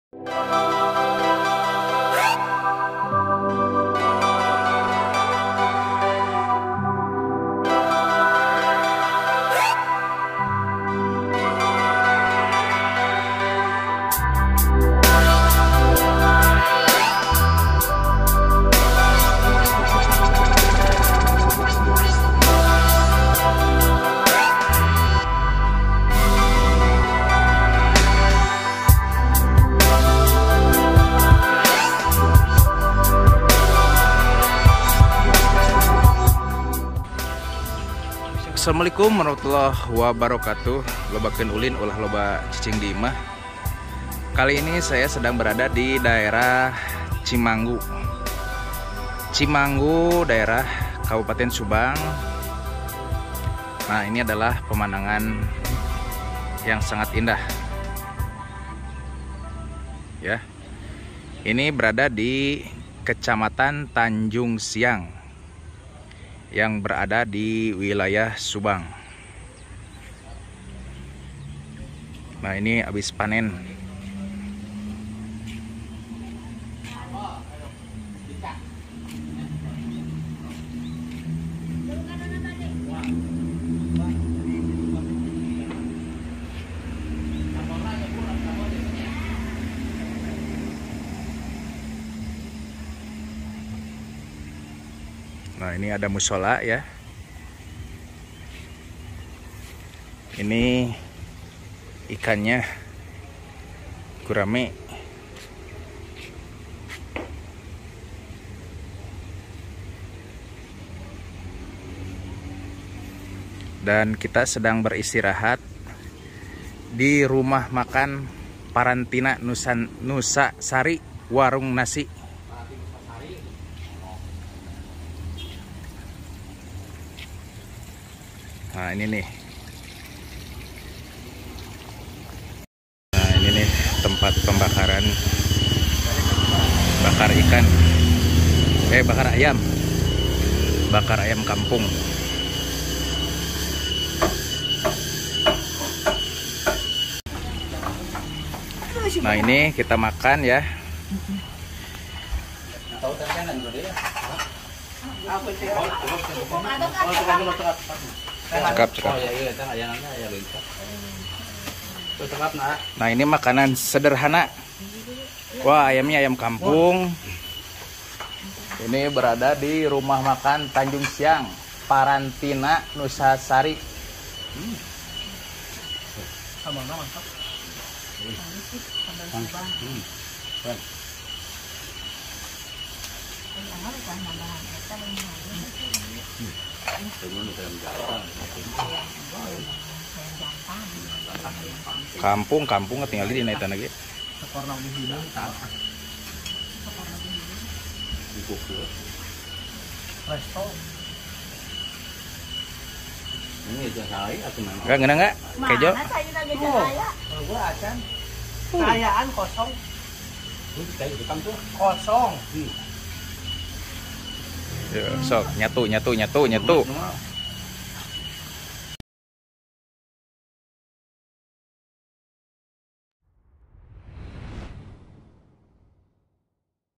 WARUNG SANGU PARANTINA NUSASARI | TANJUNG SIANG SUBANG
rumah makan yang berada di jalan tanjungsiang ,mwnyediakan ayam bakakak atau bakar ayam panggang, nikmat danpatut dicoba